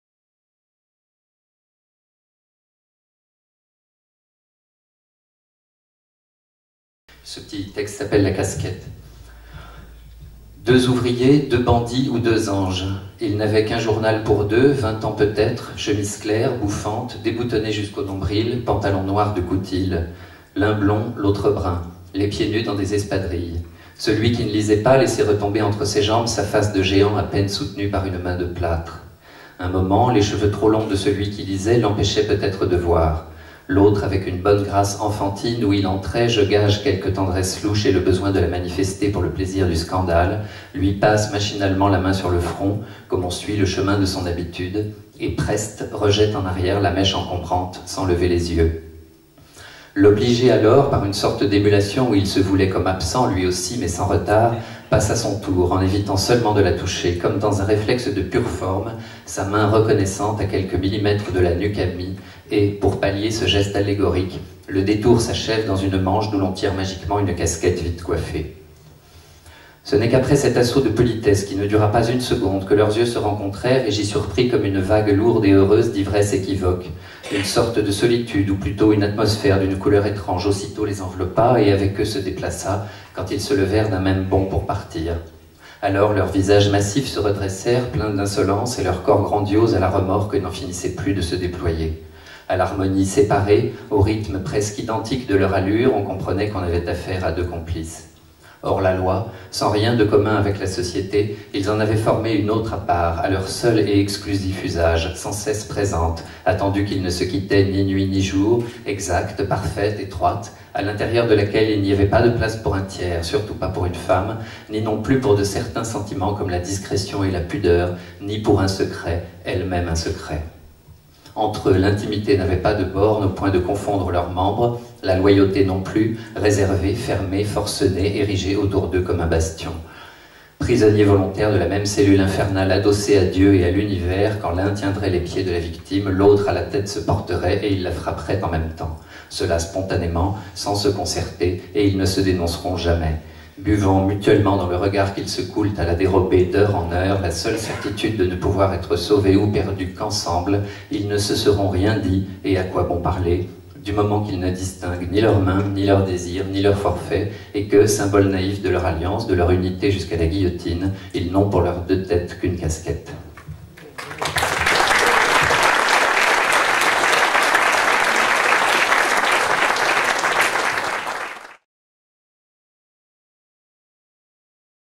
Mathieu Riboulet lit un extrait de Marcel Jouhandeau pendant les Rencontres de Chaminadour 2006
littérature, littérature française, Mathieu Riboulet, Jouhandeau, Guéret, Rencontres de Chaminadour, lecture